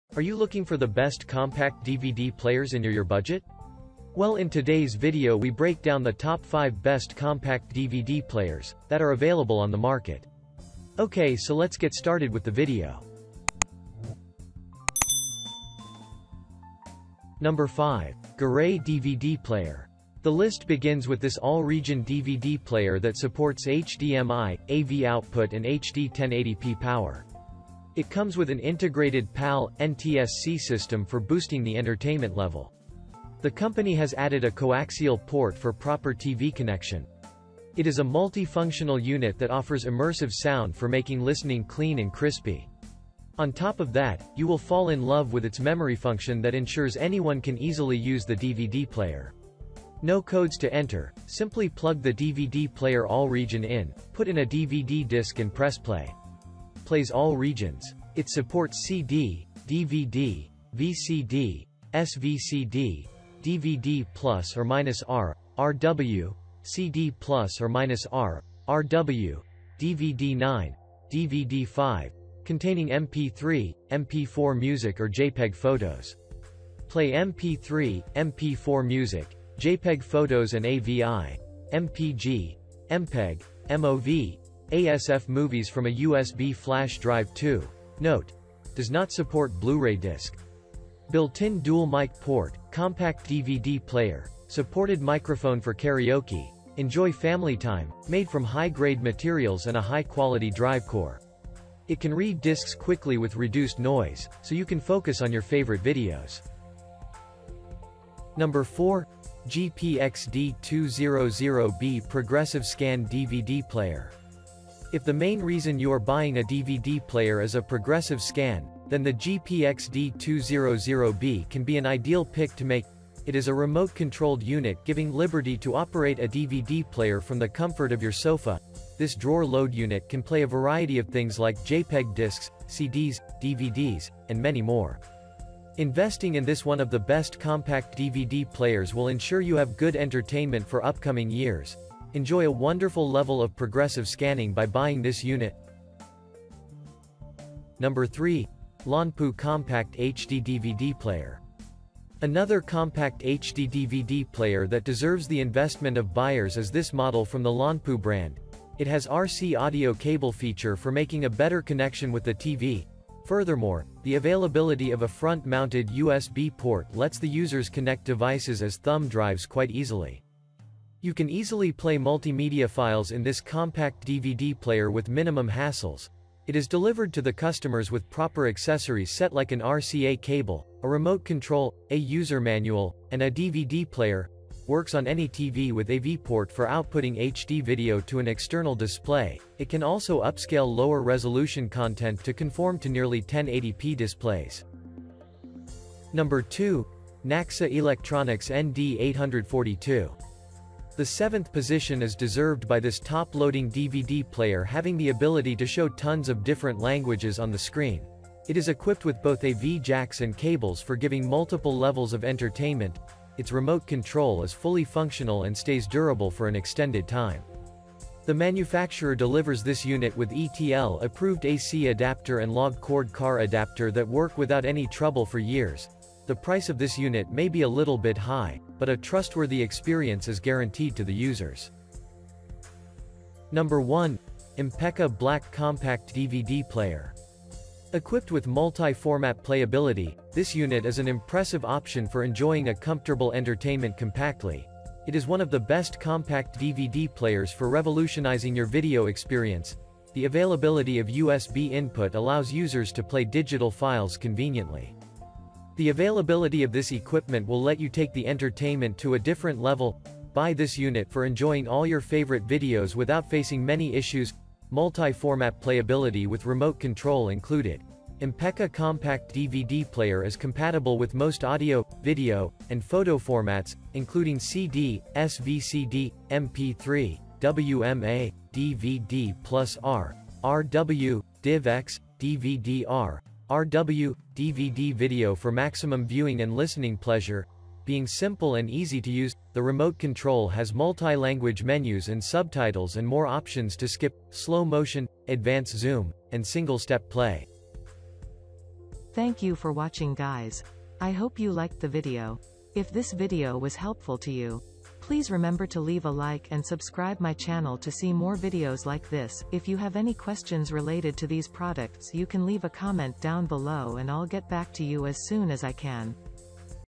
Top 5 Best Compact DVD Players in 2022
☛ All The Links to Best Compact DVD Players listed in this Video:-

►5. Gueray DVD Player.

► 4. GPX D200B Progressive Scan DVD Player.

► 3. Lonpoo Compact HD DVD Player.

► 2. NAXA Electronics ND-842.

►1. Impecca Black Compact DVD Player.